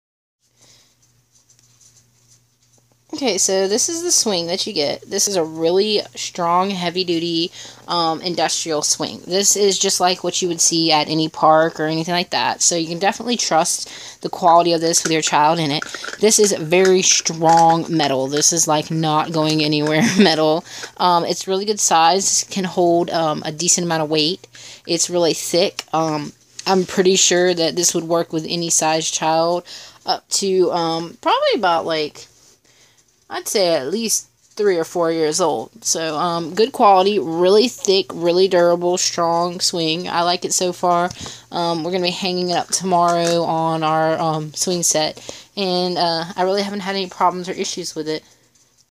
Viment High Back Full Bucket Toddler Swing Seat - YellowOffered by Viment
This Viment Full-Bucket Swing Seat is very nice to have for my son. It is very well-made nice high-quality materials that should hold up well over time and through prolonged use. This is extremely heavy duty and it is made of a nice very very thick plastic material that is extremely durable and strong I definitely trust that it will hold my child in properly during use. This does not come with the straps or chain to hang it you do need to get those yourself but it does already have the hooks to hang it attached to it. The hooks attaches are very strong and durable they're solid metal and I trust also that these two will be safe for my child and hold their weight nicely they are bolted into the plastic so they are very secure. This is just like the ones that they have on playground for children to play in so these are meant to be outside withstand any type of weather. This is a very bright yellow color which I like because I have a son it is perfect for him to use but I can also use it for my niece. All-around I think this is a really great product it is really worked well for me and my needs and I enjoy having it a lot. I have not had any problems or issues with this product so far it has been great. It is exactly what I was expecting and is accurately described in the description and photos provided.